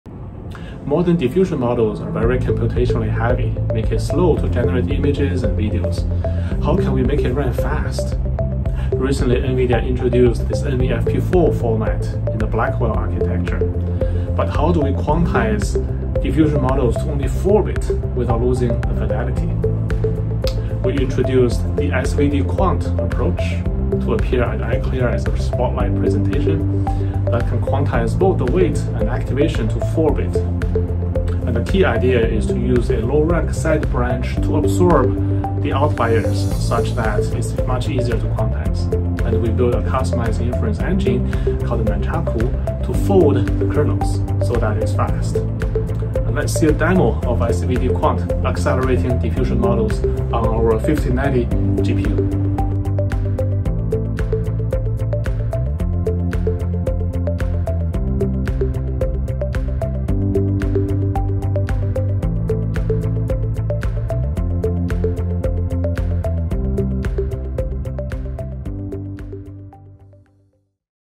SVDQuant NVFP4 Demo
SVDQuant supports NVFP4 on NVIDIA Blackwell GPUs with 3× speedup over BF16 and better image quality than INT4.